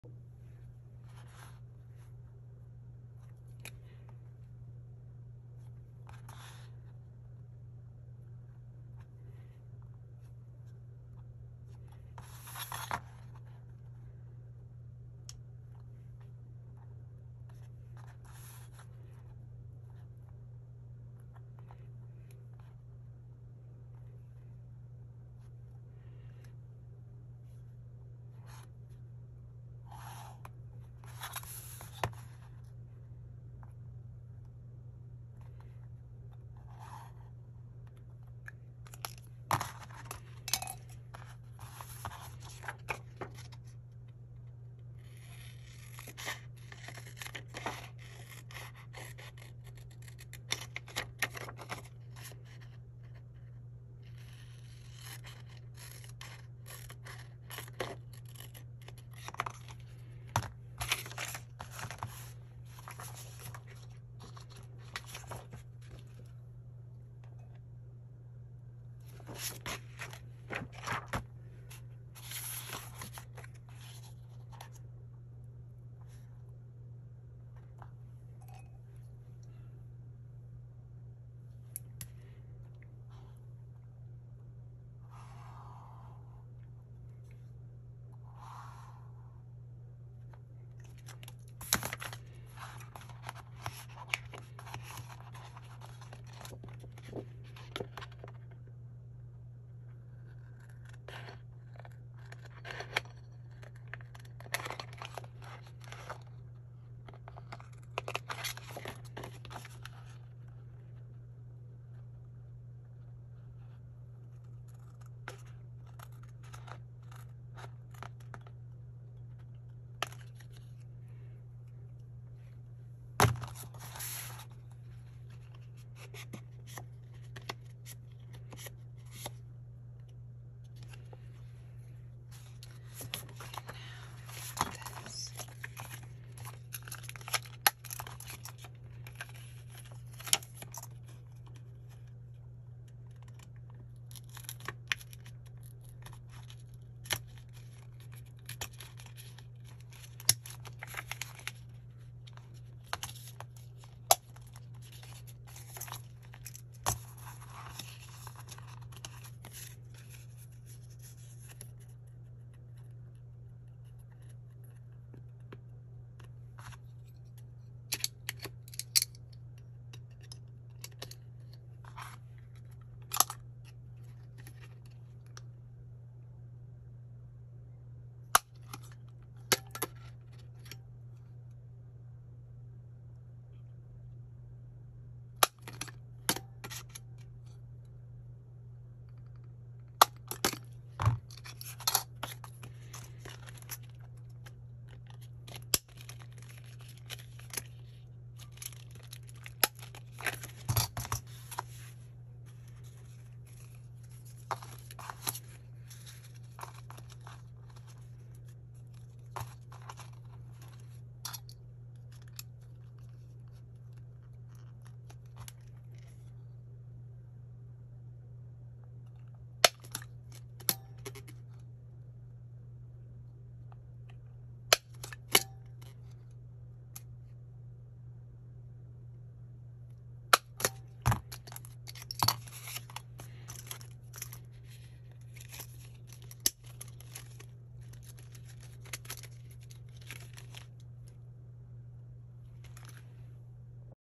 DIY Punch Guide for Mini 3 Ring Binder
Hello! This is my first step whenever I create a new tiny planner. I decide what my page size should be and make a punch guide/ page size guide out of clear plastic. The plastic lasts longer and I don’t have to worry about punching ugly double holes in my pages.

This video is most ASMR- I do whisper under my breath and one point sorry!

Page size is 2.5 inches by 3.75 inches.

3/16” hole punch:

Avery Plastic Dividers: